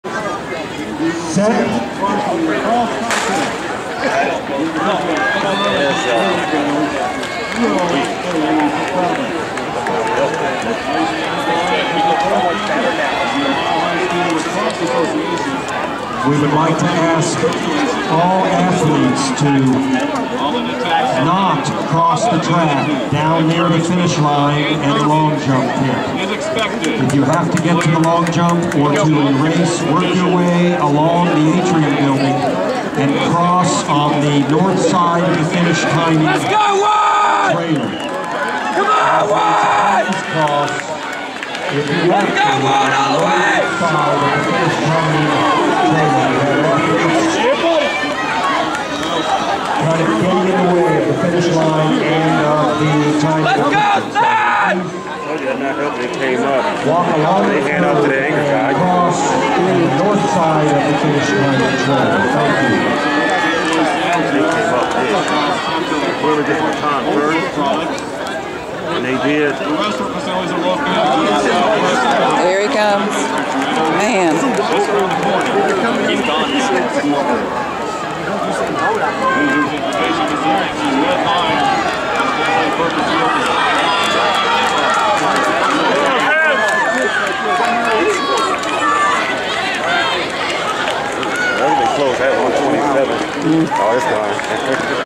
2012 Mason Track Inv - Boys 4x200m Relay (heat 1/2)
Mason 2012 Invitational: Boys 4 x 200m Relay with Mount Healthy finishing 1st in 1:27.54 minutes. Gahanna-Lincoln was 2nd with 1:29.28 min. Mason finished 5th and 6th with 1:30.32 and 1:30.55 minutes.